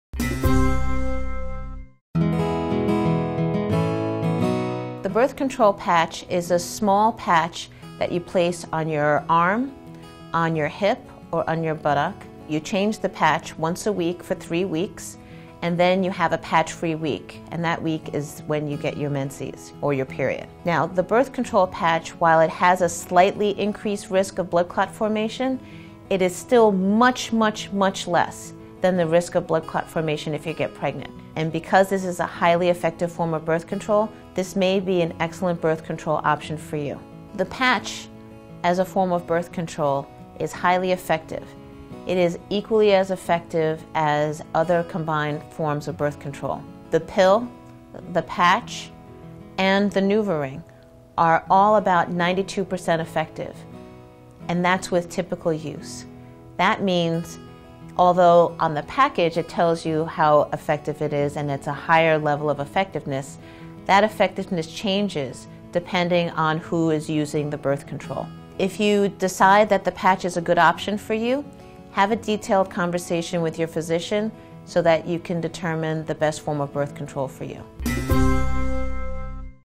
Birth Control Patch Safety & Effectiveness
Learn about the safety and effectiveness of a birth control patch in this Howcast video.

The birth control Patch is a small patch that you place on your arm, on your hip, or on your buttock. You change the patch once a week for three weeks, and then you have a patch-free week, and that week is when you get your menses or your period. Now, the birth control patch, while it has a slightly increased risk of blot clot formation, it is still much, much, much less than the risk of blood clot formation if you get pregnant. And because this is a highly effective form of birth control, this may be an excellent birth control option for you.

The patch, as a form of birth control, is highly effective. It is equally as effective as other combined forms of birth control. The pill, the patch, and the NuvaRing are all about 92% effective, and that's with typical use. That means, although on the package it tells you how effective it is and it's a higher level of effectiveness, that effectiveness changes depending on who is using the birth control.

If you decide that the patch is a good option for you, have a detailed conversation with your physician so that you can determine the best form of birth control for you.